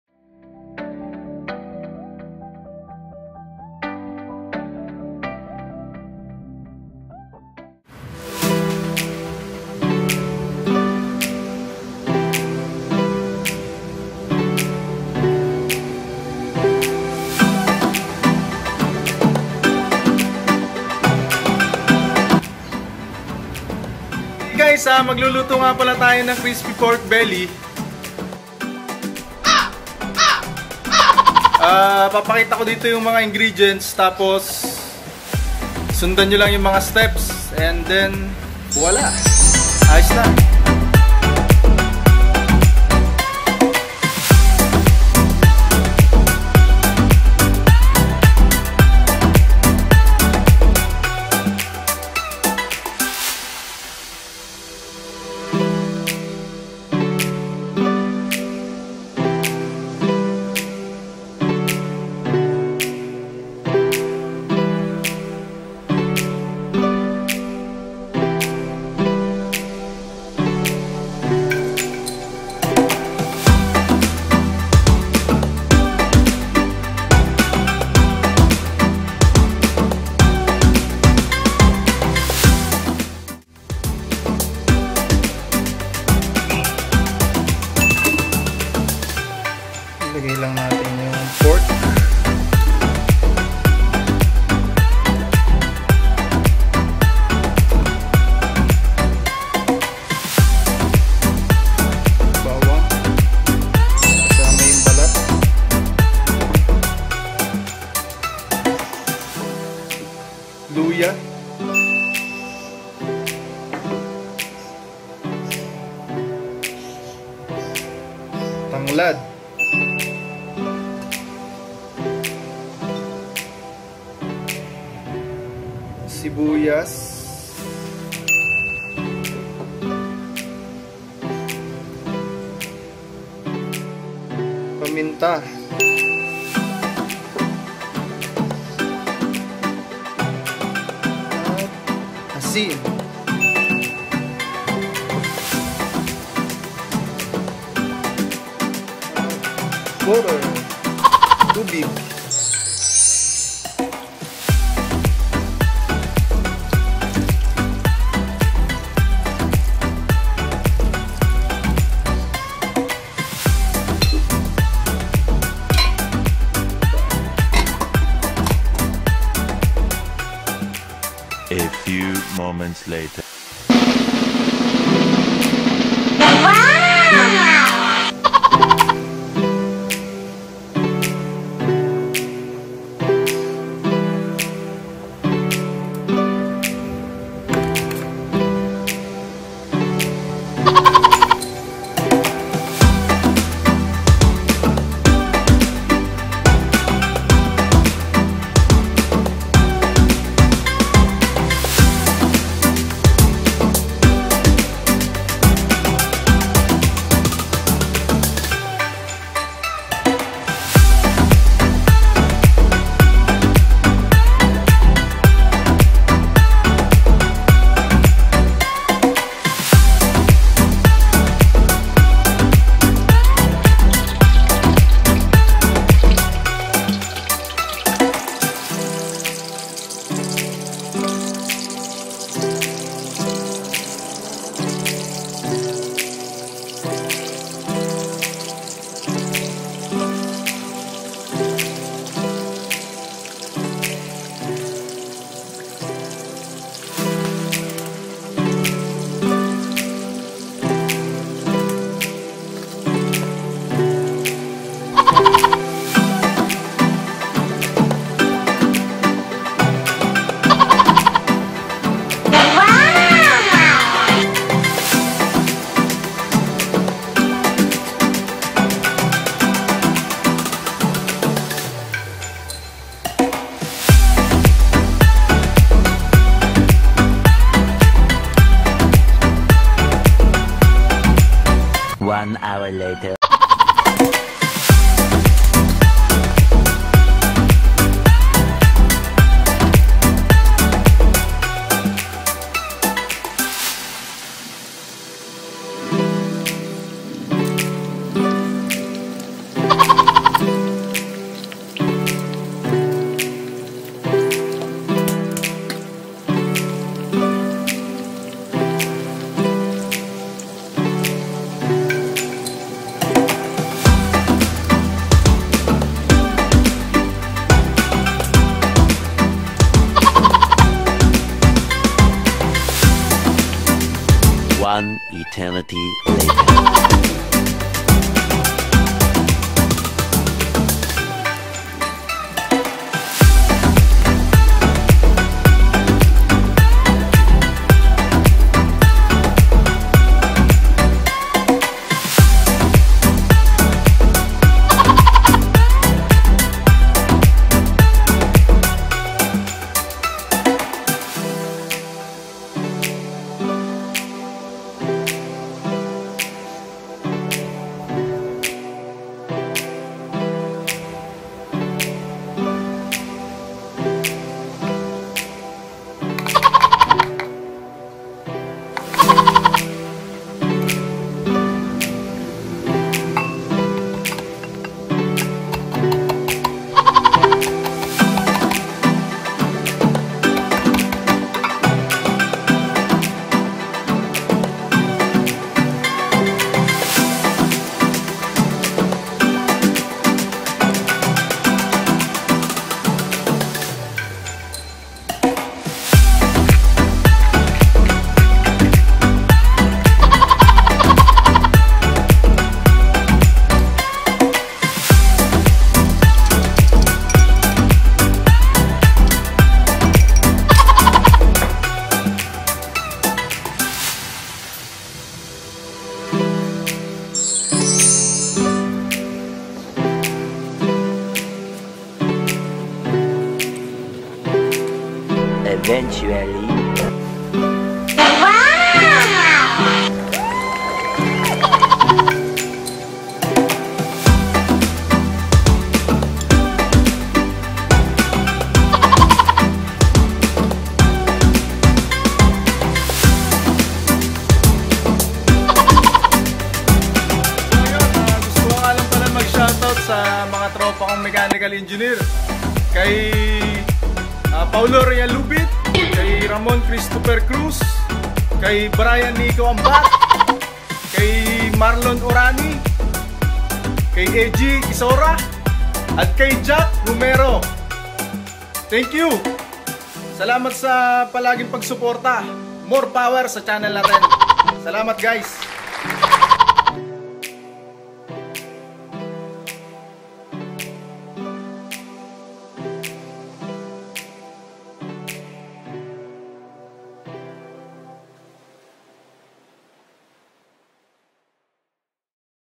HOW TO COOK CRISPY PORK BELLY | LIEMPO (Easy way)
Sawa na ba sa Ordinaryong Prito???

Manood na!!! Para malaman kung papano magluto ng Malasa at Crispy Pork Belly | Liempo

for more easy but delicious recipes!!!